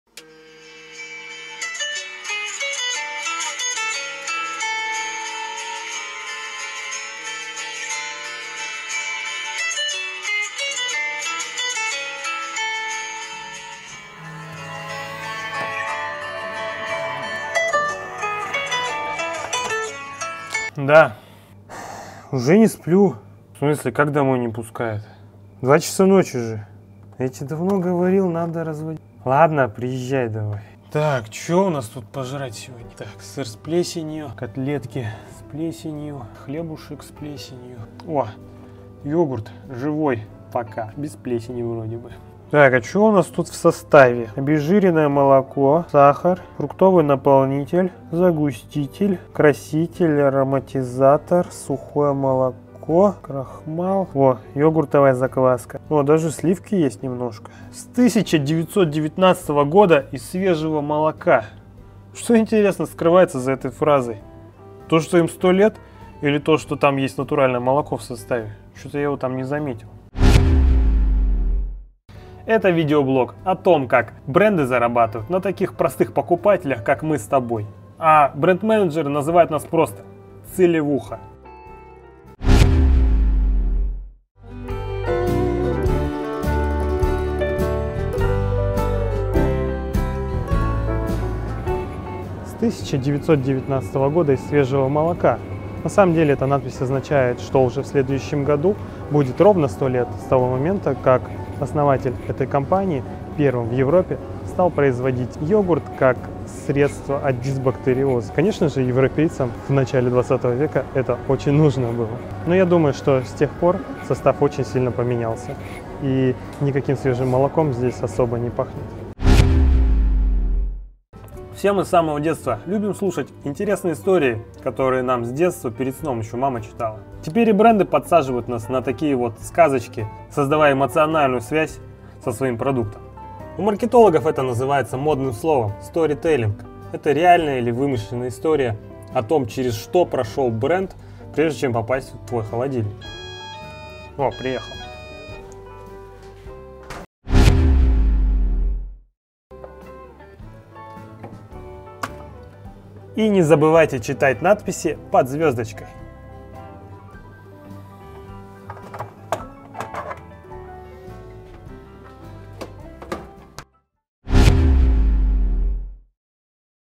Сторителлинг - просто Йогурт или целая "История"? - Product Placement
В этом выпуске я расскажу про состав йогурта со 100-летней историей, плесень в холодильнике, и почему сторителлинг (Storytelling) стал маркетинговым трендом в 2018 году.
Product Placement - канал про то, как бренды зарабатывают на простых покупателях.